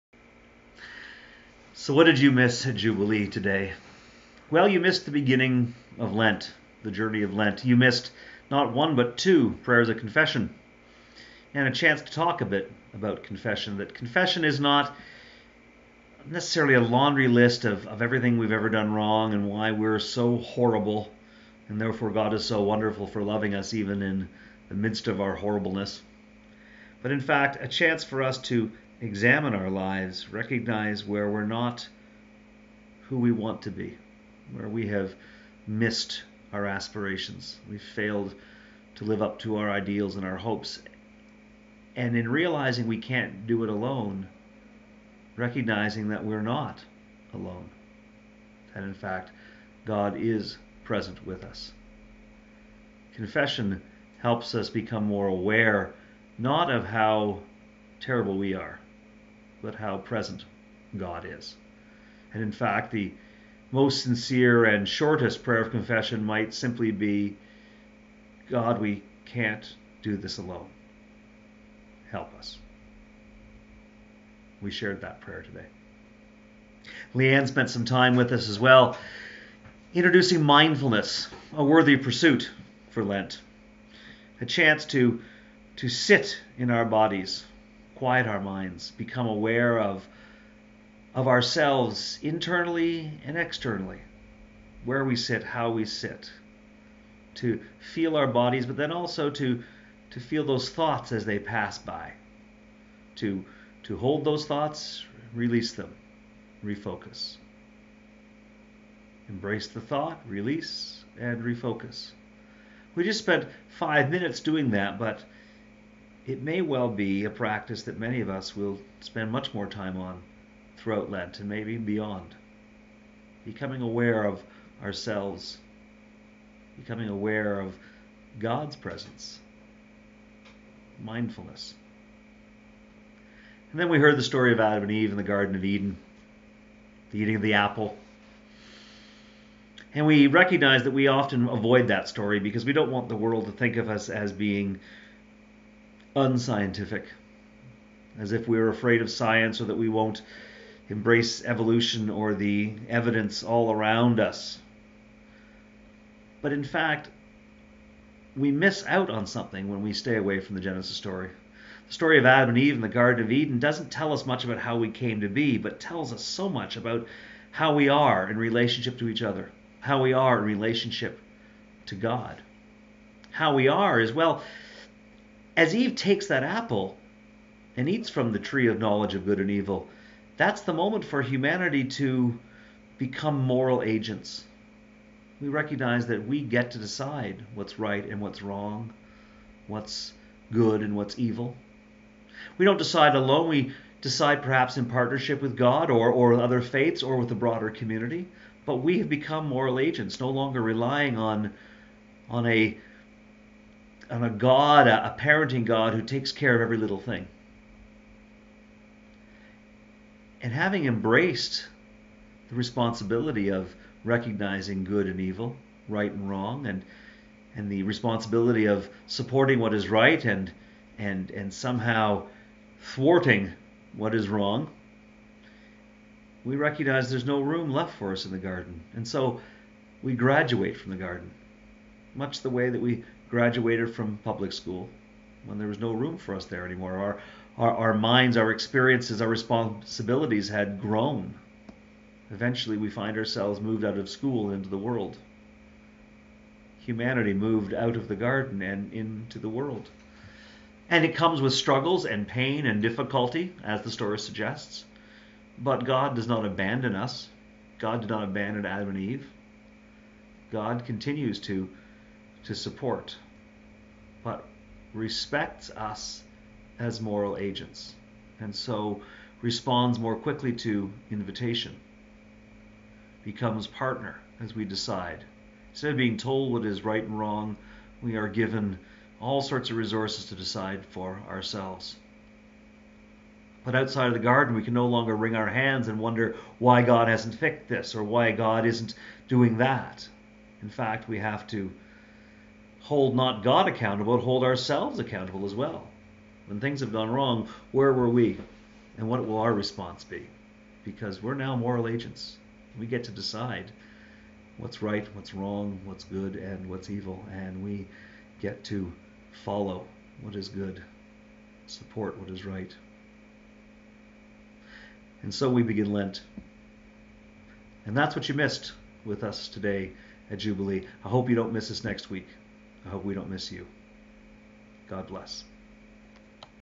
What did you miss @Jubilee, March 9, 2014
Rev. Seli sums up Sunday Worship at Jubilee United Church in just over 5 minutes!